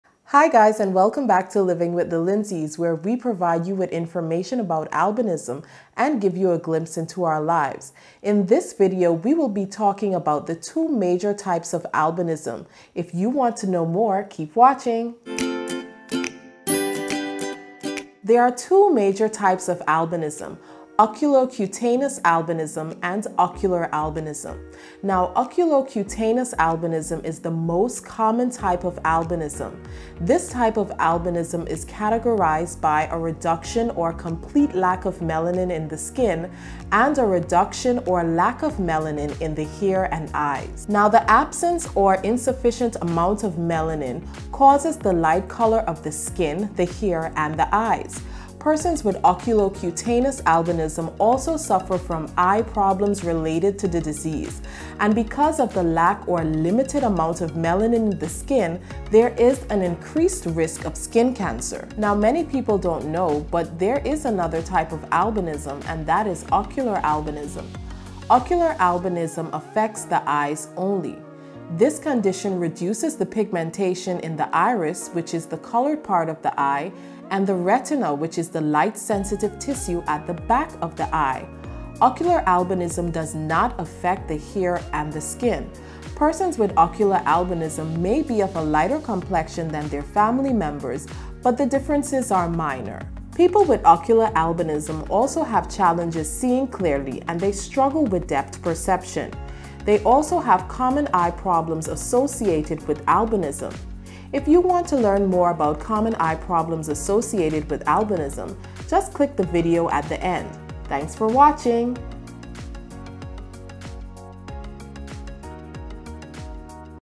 Two Major Types of Albinism
Learn about the two major types of albinism and how they affect individuals with the condition.